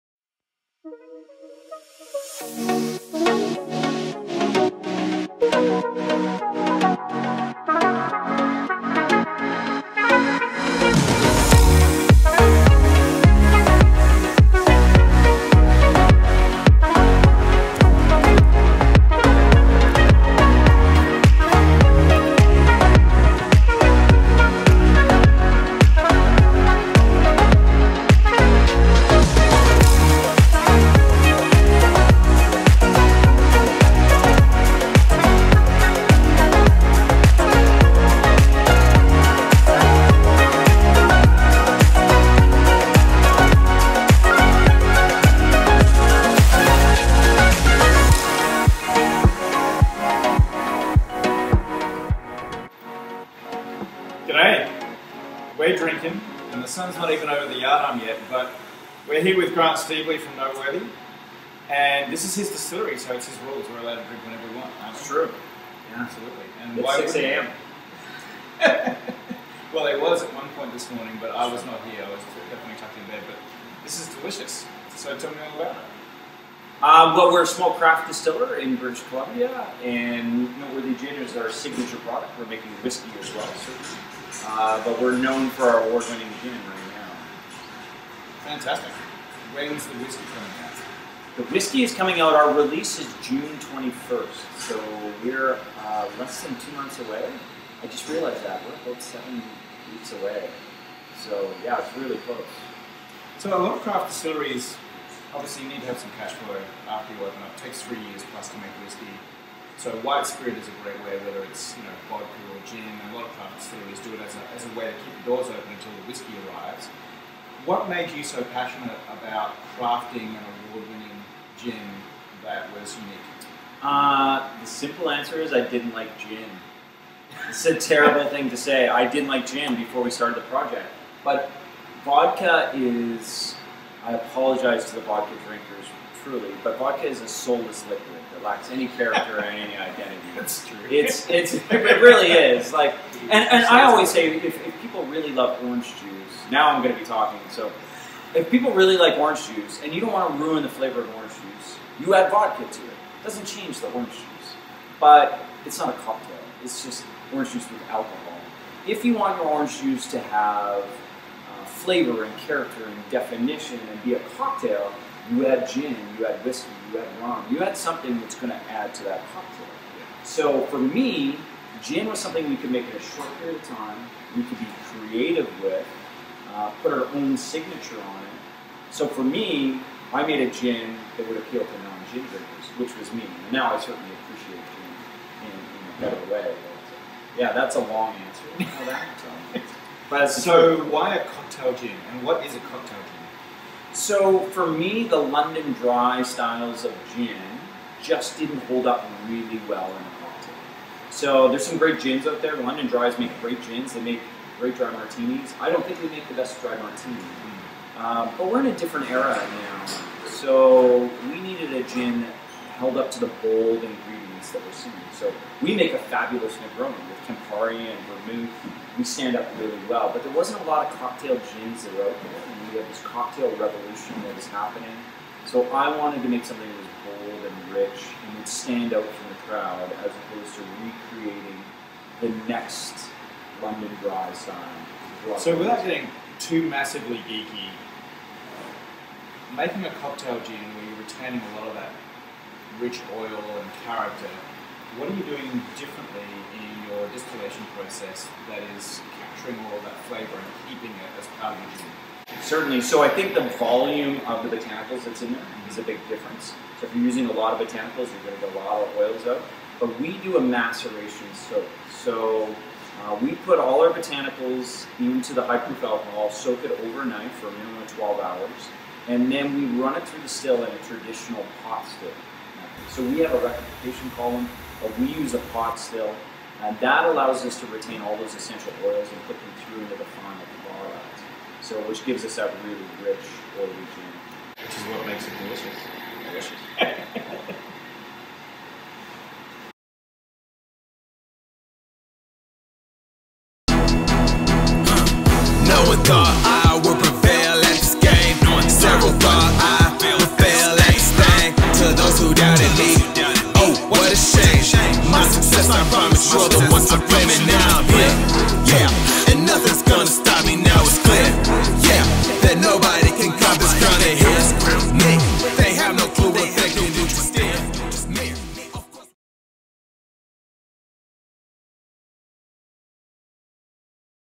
A Visit to Dubh Glas Distillery. Home of Noteworthy Gin ...and other surprises.
Jonathan, Cindy and puppy Sashes took a trip out to British Columbia to have a visit with Stevely and PJ.

Their Noteworthy Gin is winning awards and our mission was to find out what all the fuss is about.

What is their secret? (revealed at the end of the video)
Why gin?
What's with the orange?
Do you think Sashes would make a perfect distillery dog?